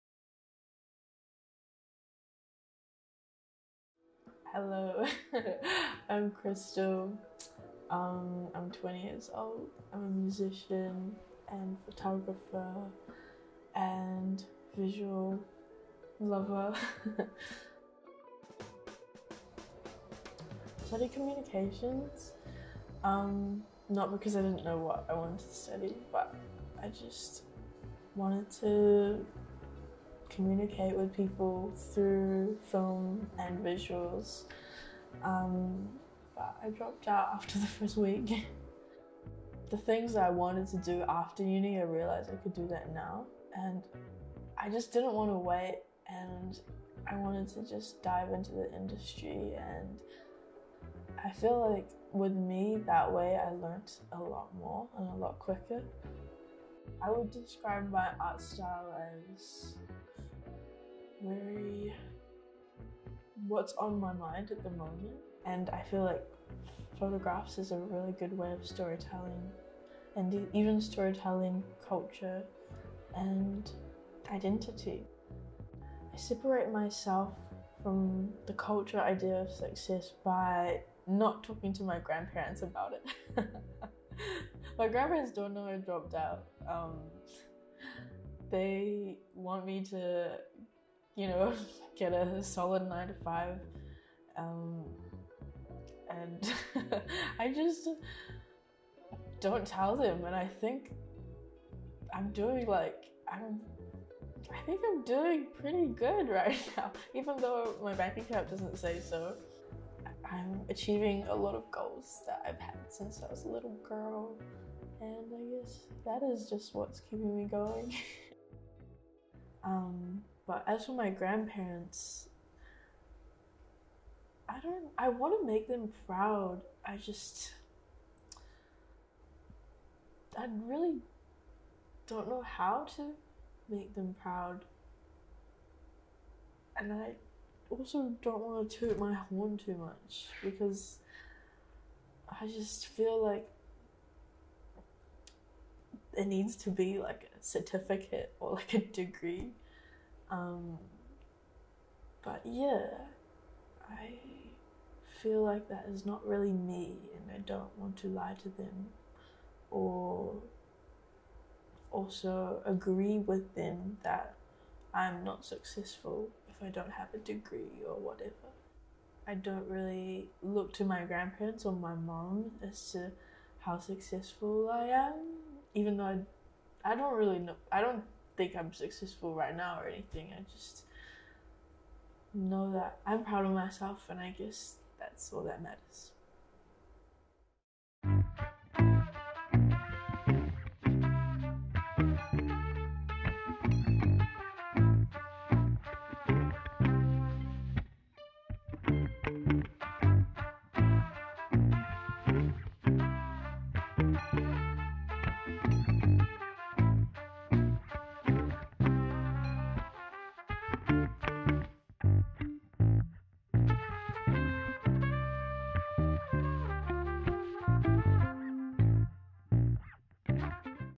Empty Red Envelope
Crystal Chen is an emerging artist in Auckland, New Zealand. She is a musician, a photographer, an all round creative. In every sense of the word Crystal is a hustler, paving her own pathway in an industry which has doesn’t typically platform Pan-Asian voices. But is diverging from the traditional pathway worth the risk?

Featuring:
Crystal Chen

Created By:
Faith Cruz, Rachel Sung, and Wanaka Yamasaki